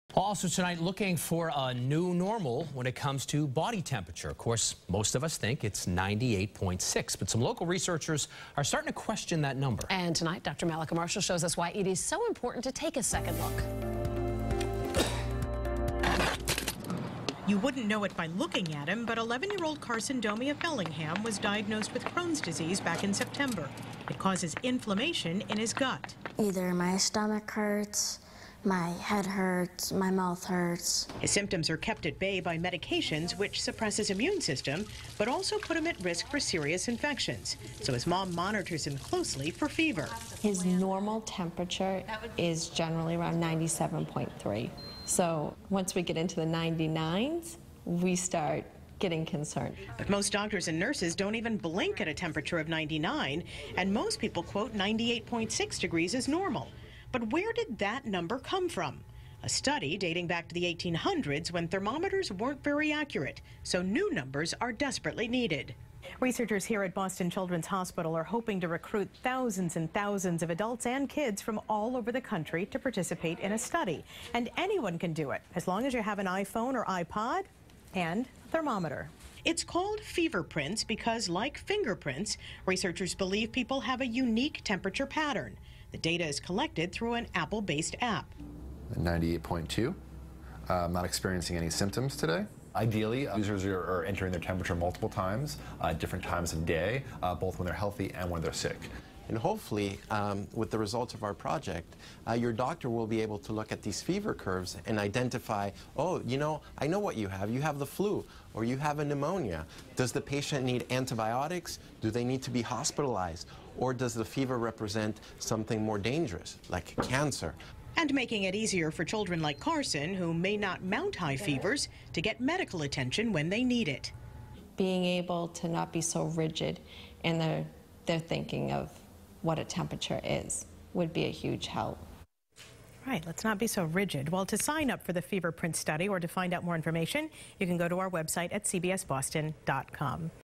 Researchers At Boston Children's To Study Body Temperatures
Boston Children’s Hospital researchers are hoping to recruit thousands of adults and kids from all over the country to participate in the "Feverprints" study.WBZ-TV's Dr. Mallika Marshall reports.